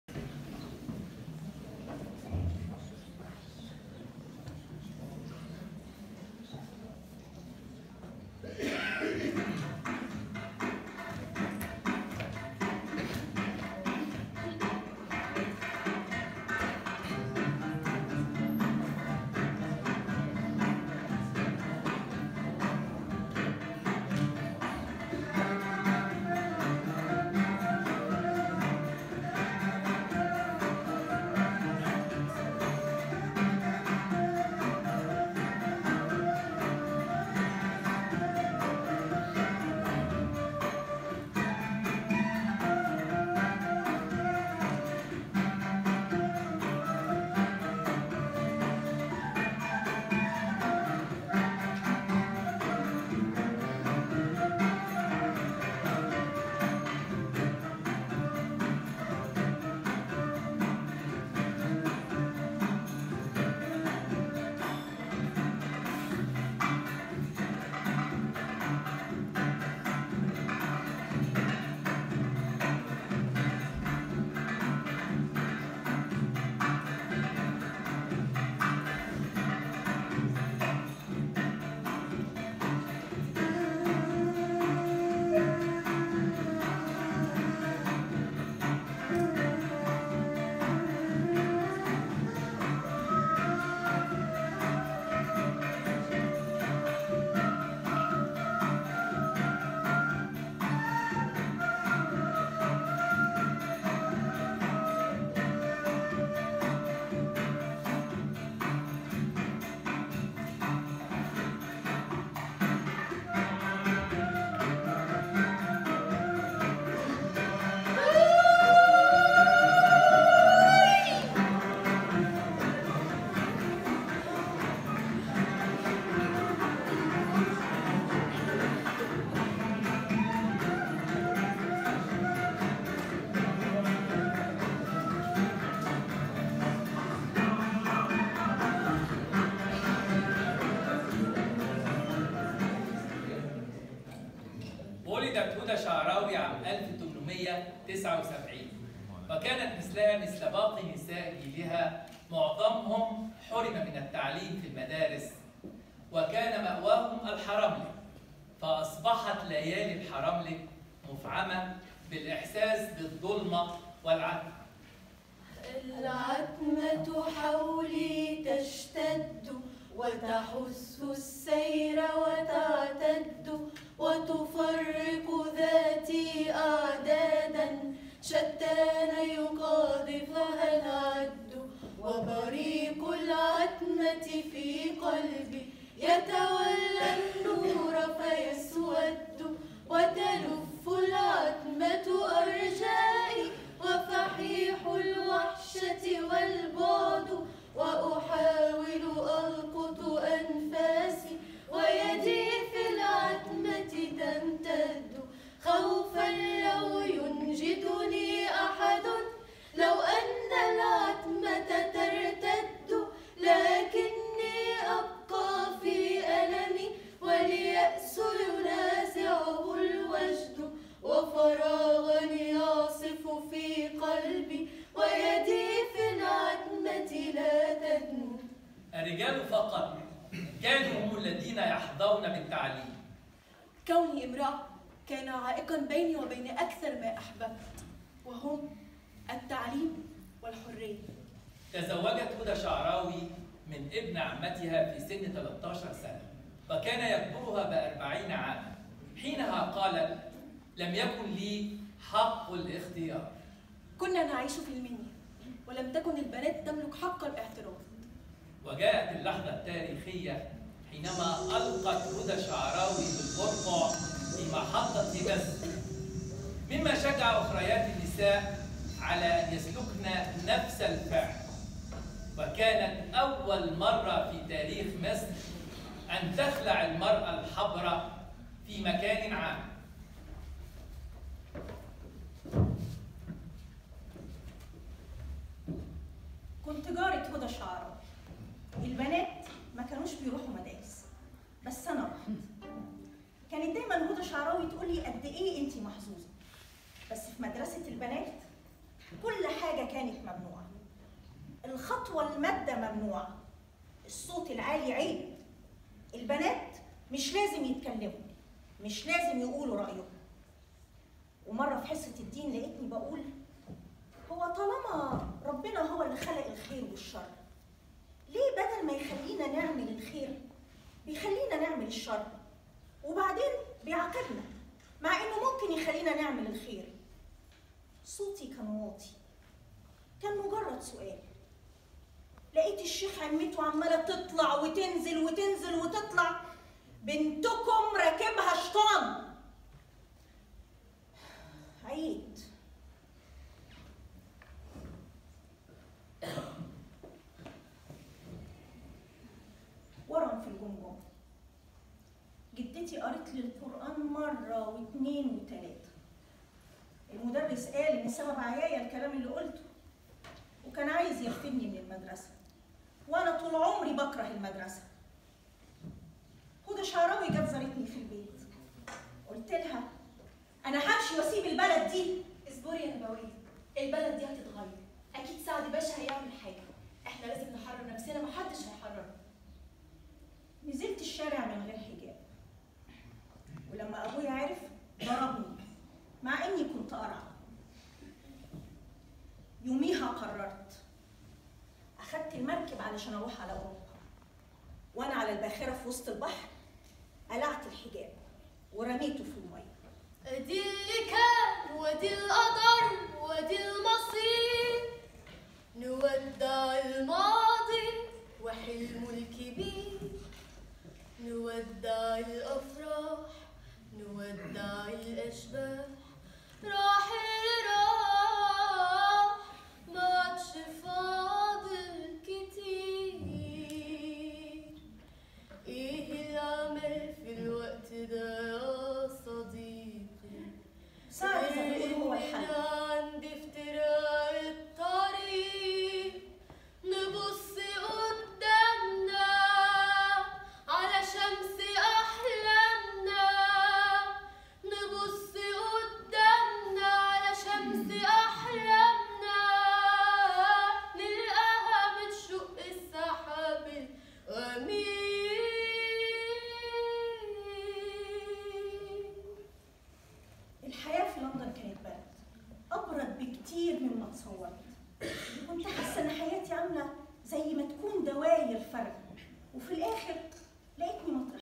"1919 Revolution: A Centennial Celebration" celebrating Hoda Shaarawy
The Space of Culture presents
"1919 Revolution: A Centennial Celebration"  theater performance about the Egyptian feminist Hoda Shaarawy,  at Heliopolis University for Sustainable Development
Narrator & Actor:  Hamada Shousha
Actresses: Sofia Rasmy, Nancy El Hadidy, Walaa Ahmed